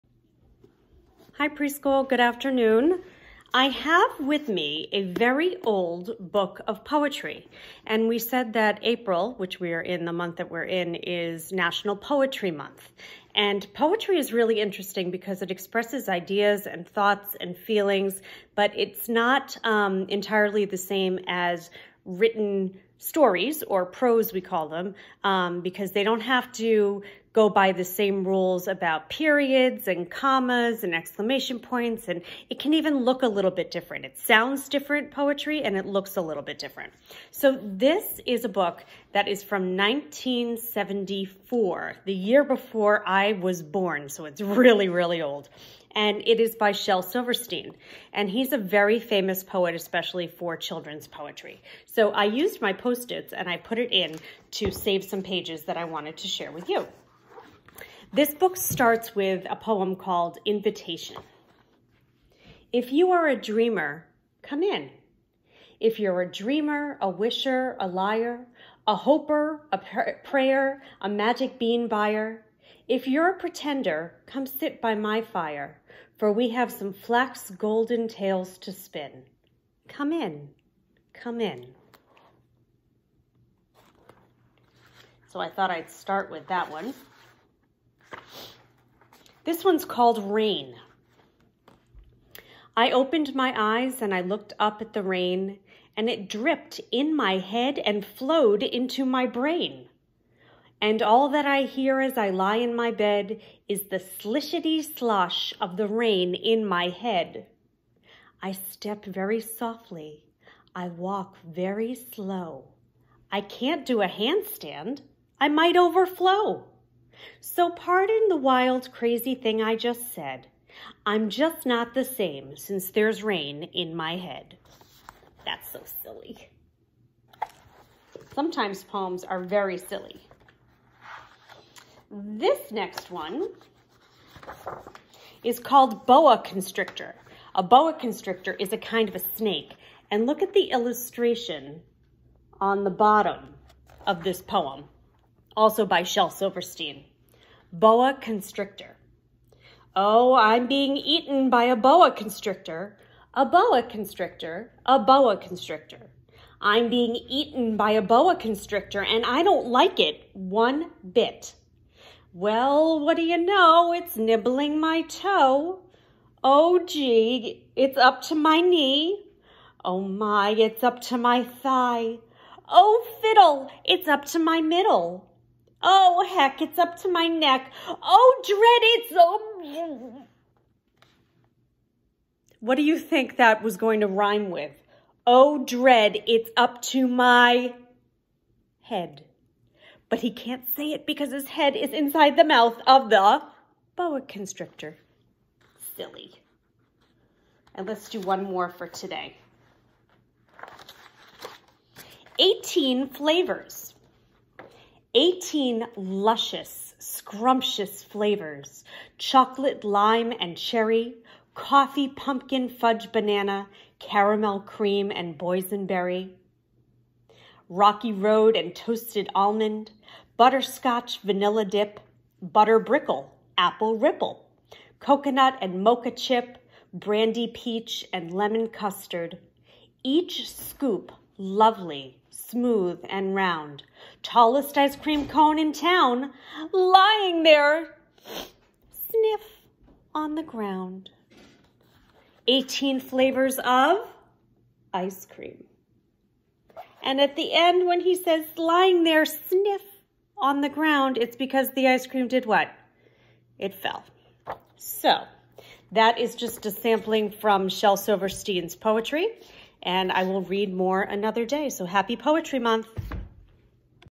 Poetry Read Aloud
Poems from Shel Silverstein’s Where The Sidewalk Ends